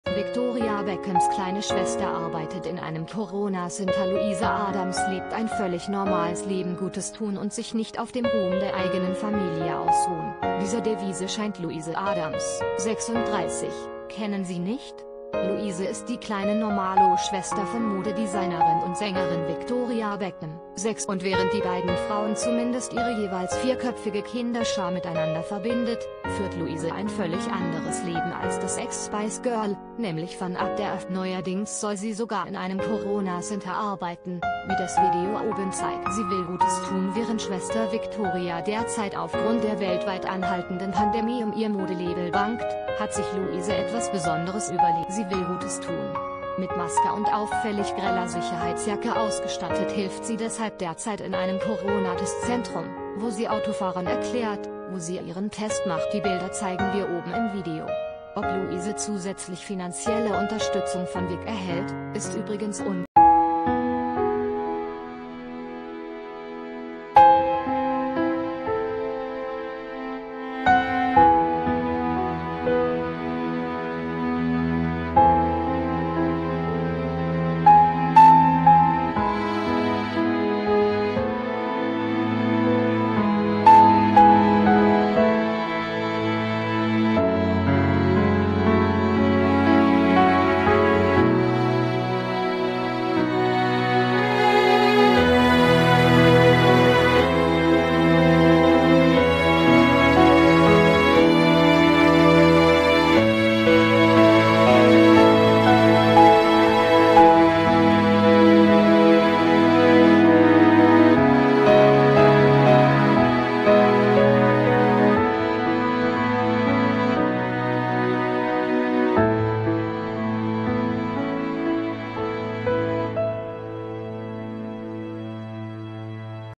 Victoria Beckhams kleine Schwester arbeitet in einem Corona-Center
Victoria Beckhams kleine Schwester arbeitet in einem Corona-Center  Louise Adams lebt ein völlig normales Leben    Gutes tun und sich nicht auf dem Ruhm der eigenen Familie ausruhen – dieser Devise scheint Louise Adams (36) zu folgen. Kennen Sie nicht? Louise ist die kleine „Normalo“-Schwester von Modedesignerin und Sängerin Victoria Beckham (46). Und während die beiden Frauen zumindest ihre jeweils vierköpfige Kinderschar miteinander verbindet, führt Louise ein völlig anderes Leben als das Ex-„Spice Girl“ – nämlich fernab der Öffentlichkeit. Neuerdings soll sie sogar in einem Corona-Center arbeiten, wie das Video oben zeigt.  Sie will Gutes tun   Während Schwester Victoria derzeit aufgrund der weltweit anhaltenden Pandemie um ihr Modelabel bangt, hat sich Louise etwas besonderes überlegt. Sie will Gutes tun. Mit Maske und auffällig greller Sicherheitsjacke ausgestattet hilft sie deshalb derzeit in einem Corona-Testzentrum, wo sie Autofahrern erklärt, wo sie ihren Test machen können. Die Bilder zeigen wir oben im Video. Ob Louise zusätzlich finanzielle Unterstützung von Vic erhält, ist übrigens unklar.